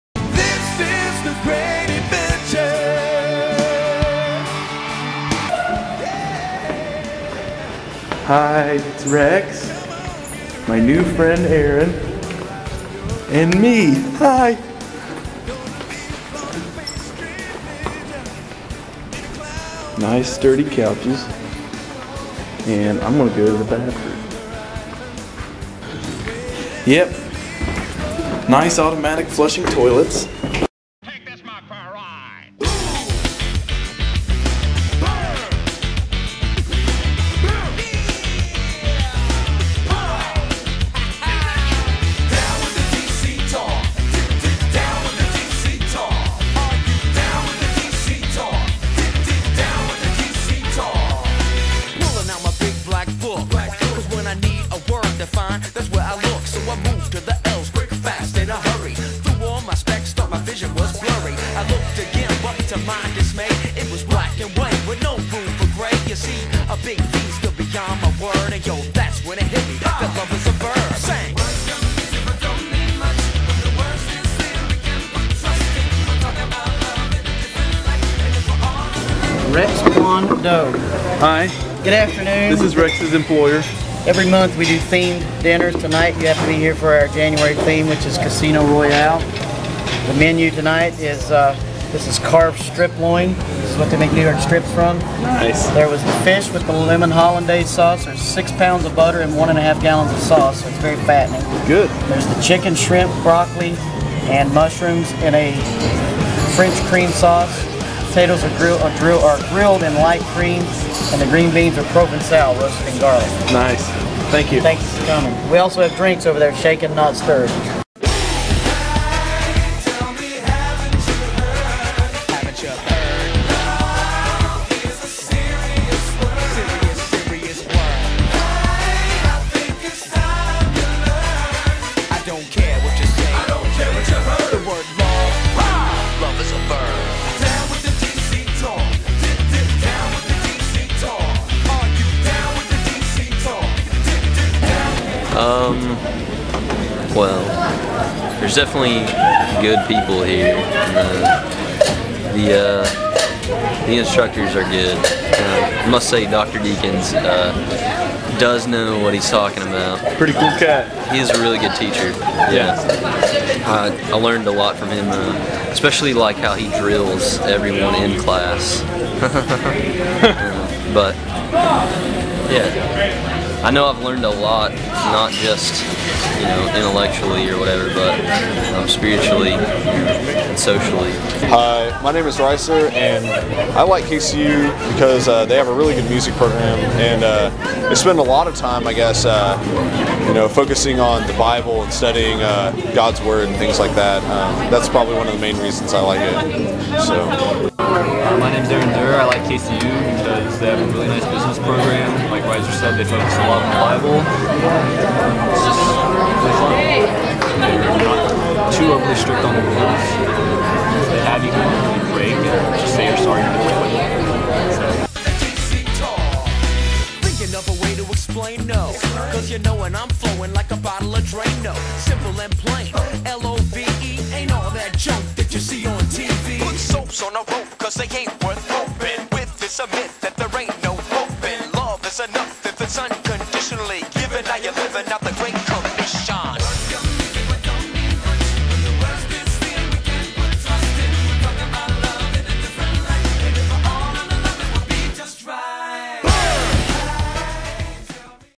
Rick's Trip to KCU
Rick's trip to Kentucky Christian University to hang out with Rex and lead worship at The Well.